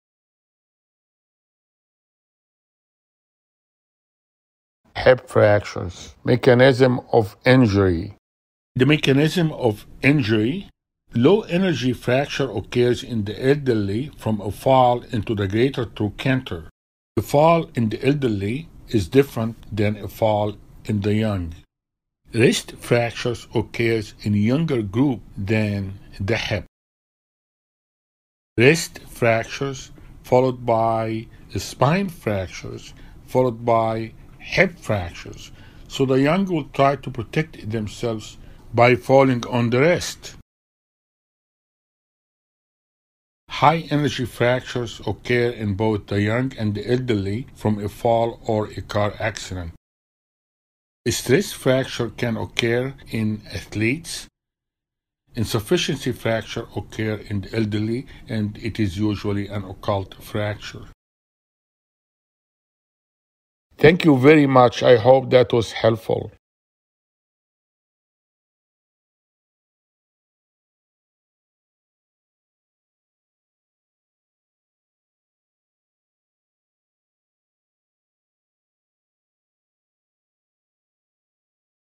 Hip Fractures Mechanism Of Injury - Everything You Need To Know - Dr. Nabil Ebraheim
Dr. Ebraheim’s educational animated video describes hip fractures mechanism of injury.

Hip Fractures Mechanism of Injury
Low energy fracture occurs in the elderly from a fall onto the greater trochanter. The young will try to protect themselves by falling on the wrist. High energy fractures can occur in both the young or the elderly  form either a fall or a car accident.